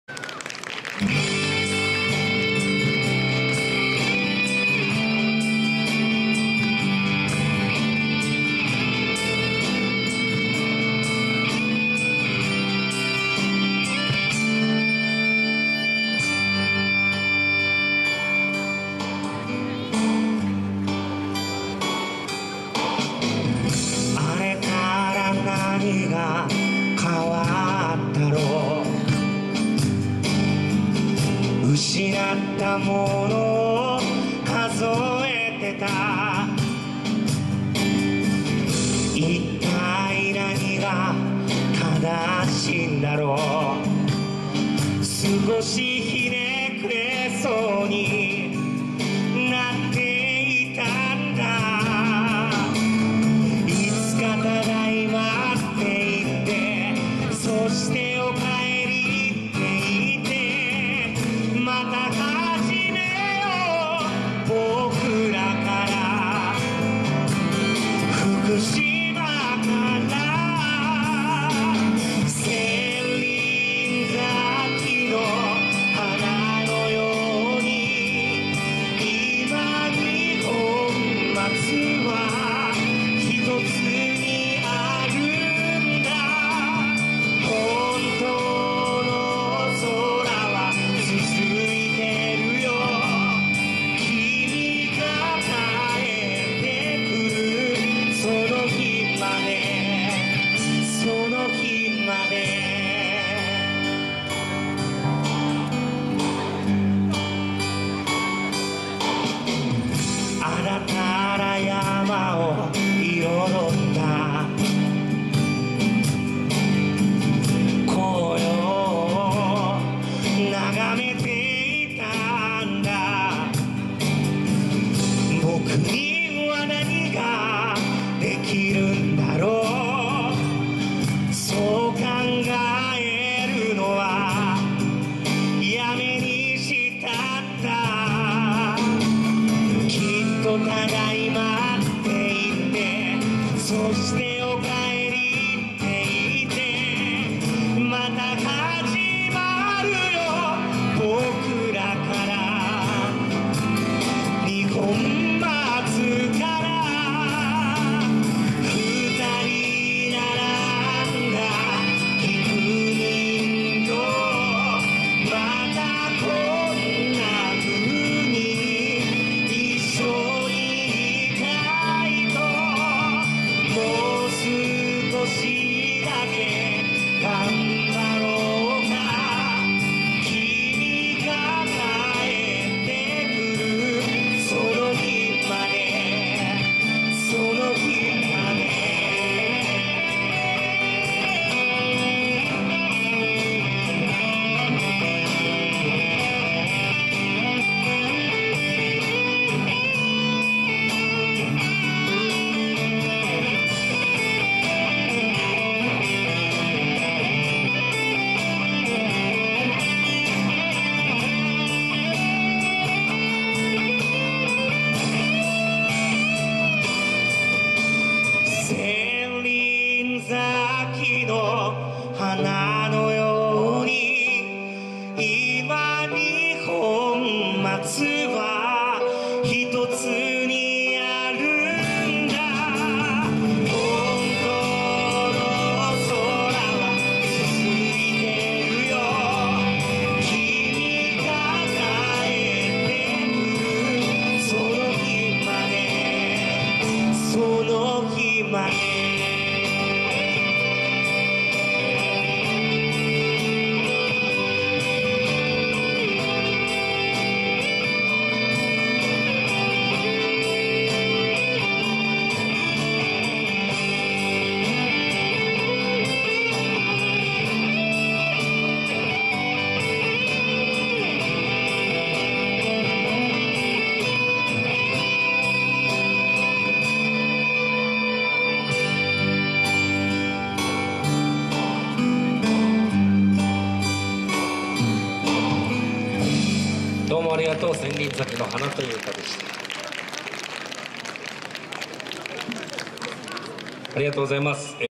③福島の元気をaveさんから 棚倉城跡にて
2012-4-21　棚倉城跡にて③
元気なaveさん、手持ちvideoカメラでしたが　真正面のシートに座り
膝の上に載せて　みかげ撮影です